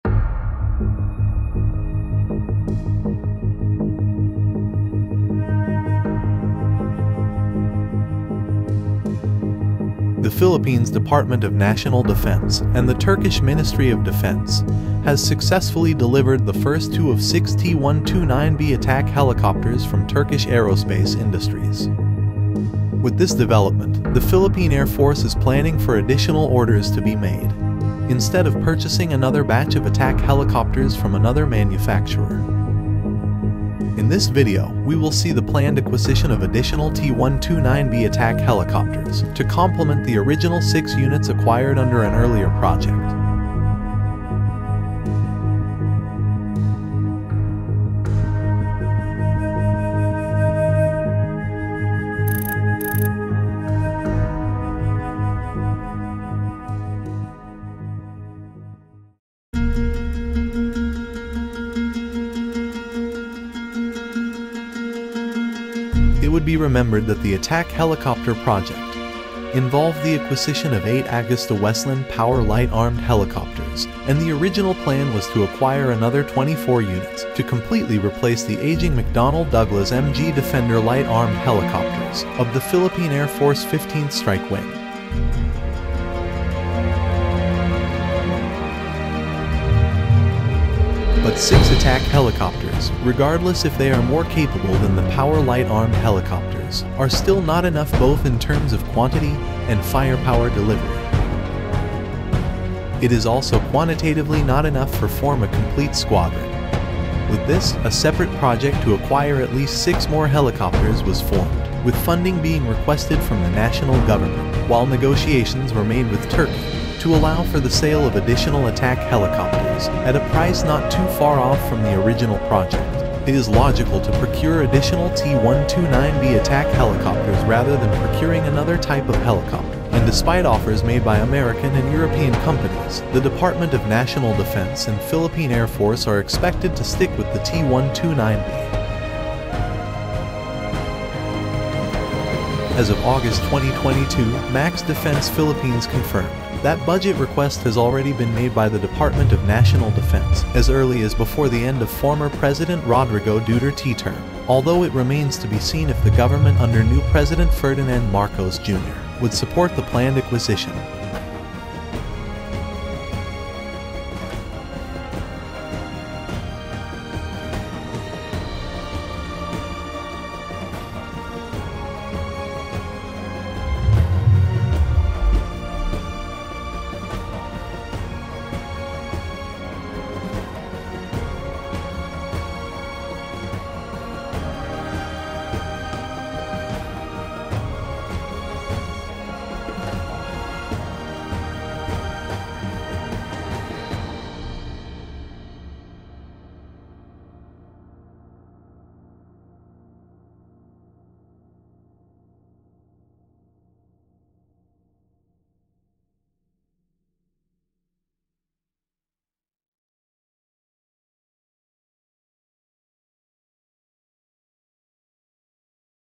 12 Units of T129B Attack Helicopters for Philippine Air Force | PrimeCheck
The Philippine Air Force (PAF) is planning for additional orders to be made of 6 units of T129B attack helicopters this will make 12 units of T129B in its fleet.

Source: PH Army, DND, MaxDefense